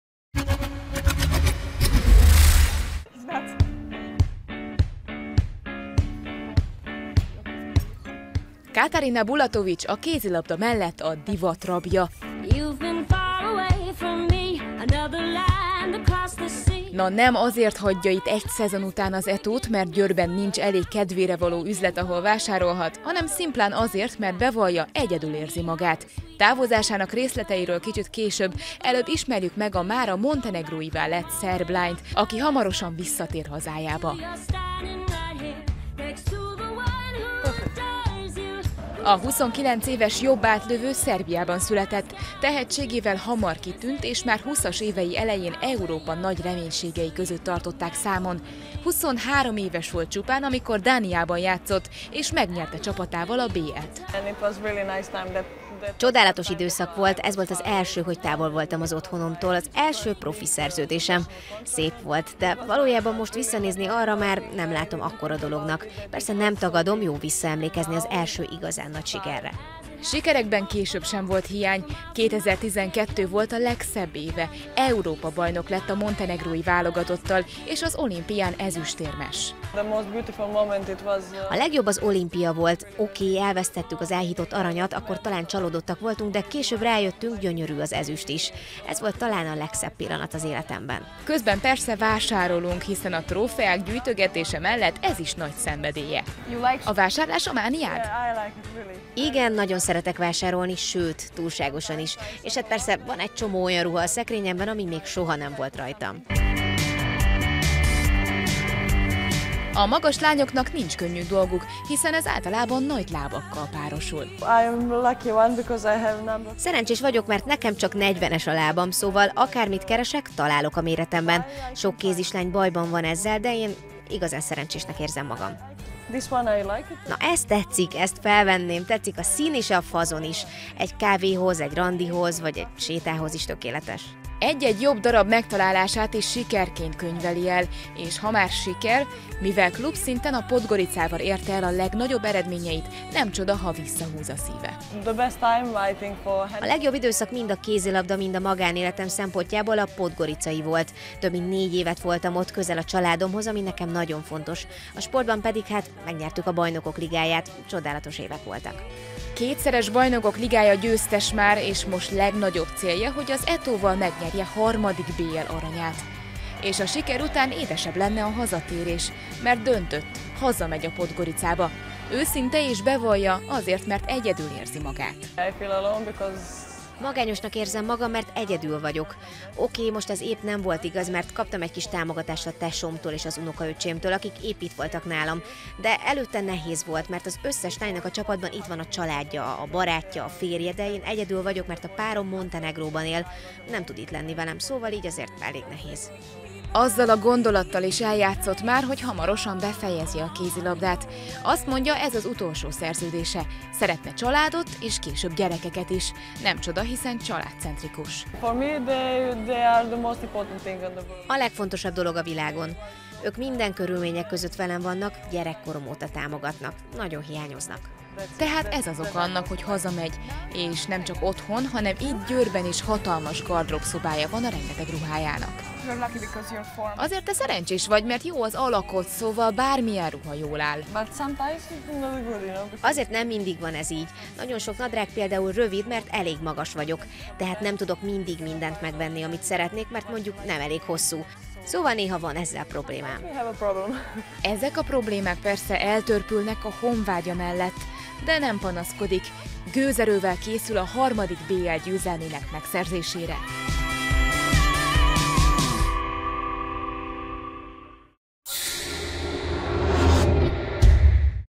Katarina Bulatovic, a trendmániás
A kétszeres BL-győztes Katarina Bulatovic a harmadik Bajnokok Ligája-trófea megszerzésére készül a címvédő Győri Audi ETO KC-vel, aztán nyáron hazatér a Buducsnoszt Podgoricába. A divatot, a szép ruhákat és a vásárlást imádó Katjával egyik kedvenc hobbijához csatlakoztunk, shoppingoltunk és közben persze hazamenetele okairól és jövőbeli terveiről is beszélgetett Szabados Ágival.